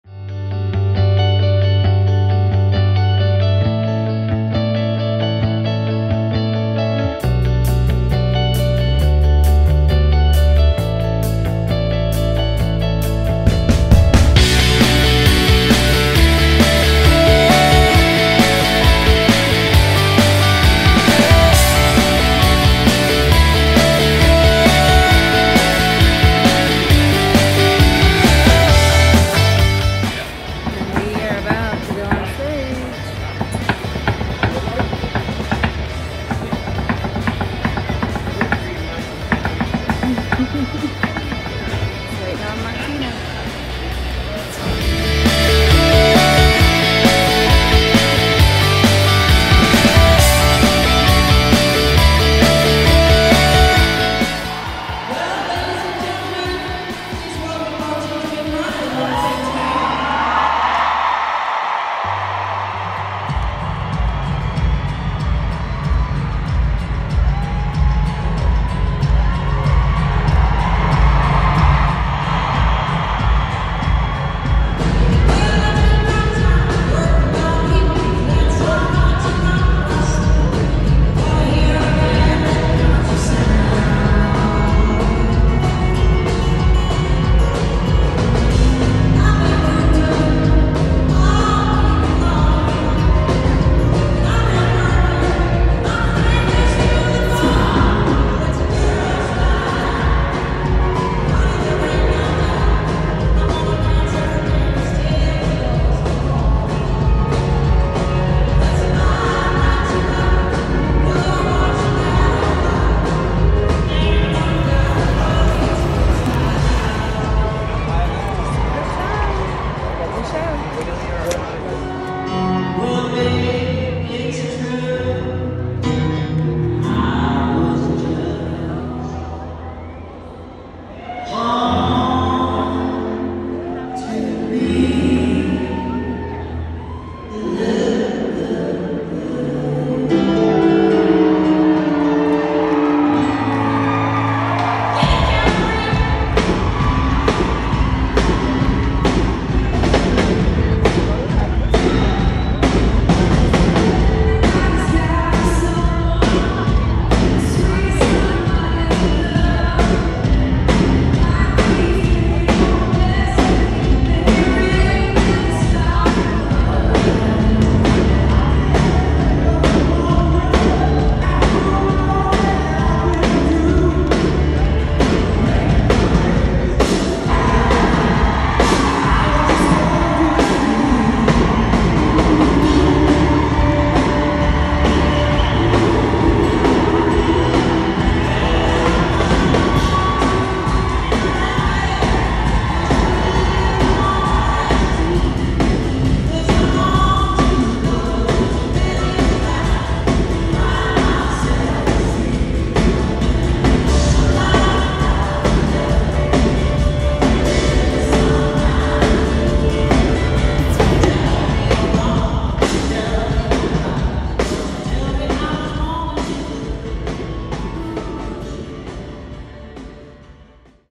Judds Final Tour with Martina McBride & Little Big Town Feb 9-11 2023
A little bit of the behind the scenes and parts of the show this particular weekend on the Judd's Final Tour with Wynonna, Martina McBride & Little Big Town. I sang harmonies for Martina. We had a blast. We watched Wy's show every night it was so good. I'm a huge Judd's fan, so this was a wonderful tour to be on. I have sung The Judd's music since I was a teenager. Who would have thought that 36 years later I would be out on the road with Wynonna. Dreams can happen folks - so keep dreaming!